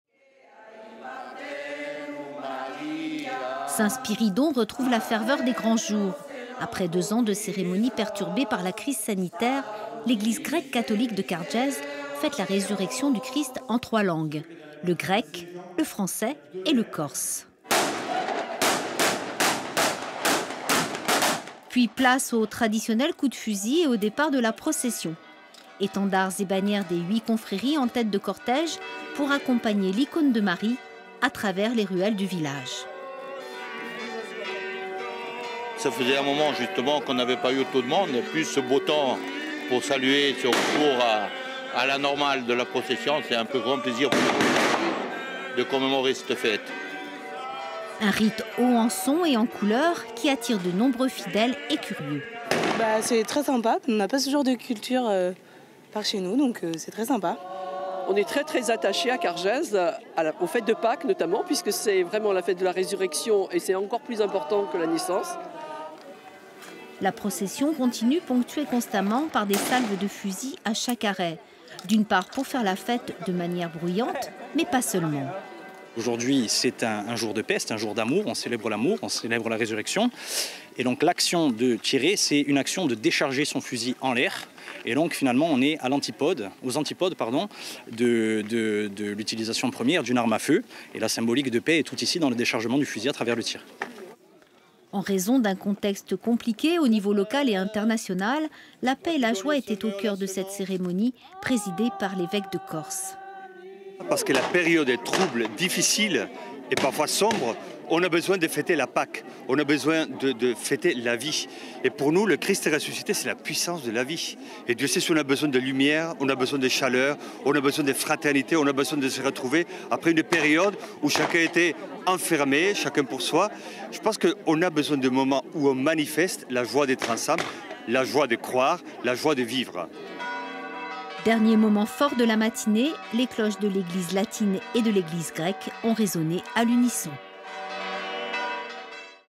Ce matin Cargese fêtait la résurrection du Christ selon le rite byzantin.
Ce matin Cargese fêtait la résurrection du Christ selon le rite byzantin ... Une cérémonie présidée par l'évêque de Corse Mgr Francois Xavier Bustillo qui a célébré la messe avec l'archimandrite de cargese le père Tony avant le départ de la procession... Un moment de joie dans les rues de la commune ponctuée par les cantiques et les traditionnels tirs de fusil

Equipe - WOLINSKY Sylvie / ROCHE Frédéric / BENARD Yann